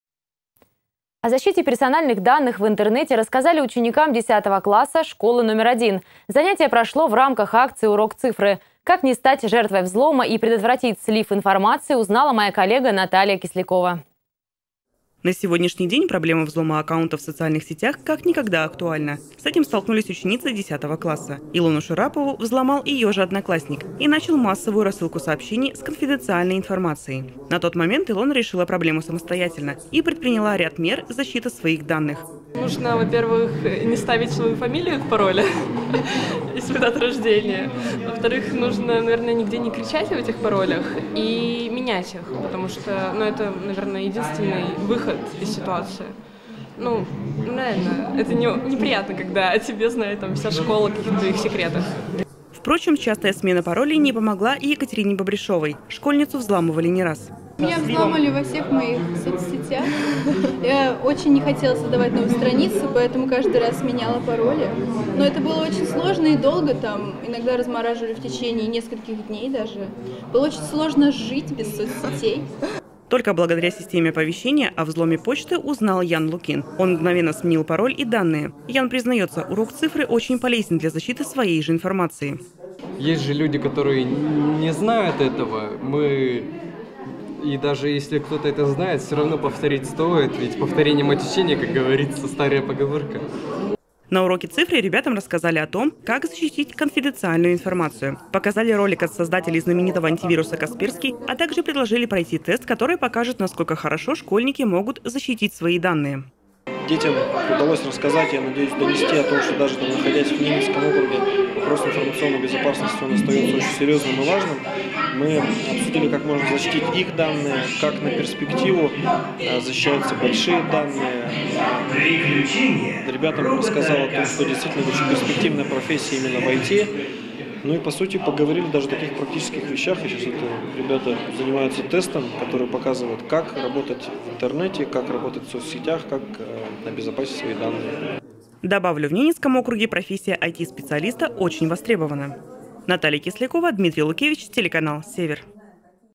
Окружным школьникам рассказали о защите персональных данных в интернете
О защите персональных данных в интернете рассказали ученикам 10 класса школы №1. Занятие прошло в рамках акции «Урок цифры». Школьники получили информацию о том, как не стать жертвой взлома и предотвратить «слив» информации.